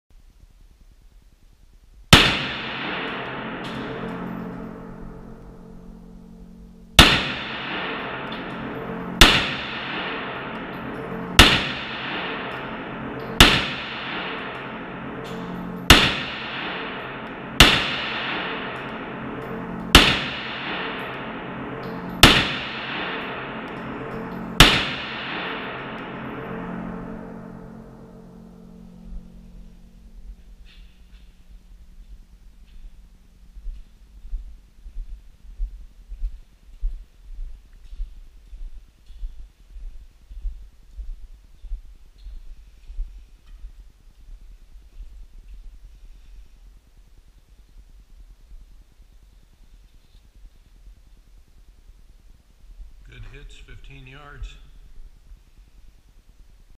Glock 17 with Crimson Trace Laser at the range
Glock 17 with Crimson Trace Laser at the range.  Just at the indoor range having some target practice at 15 yards.  Using a Glock G17, and Crimson trace laser.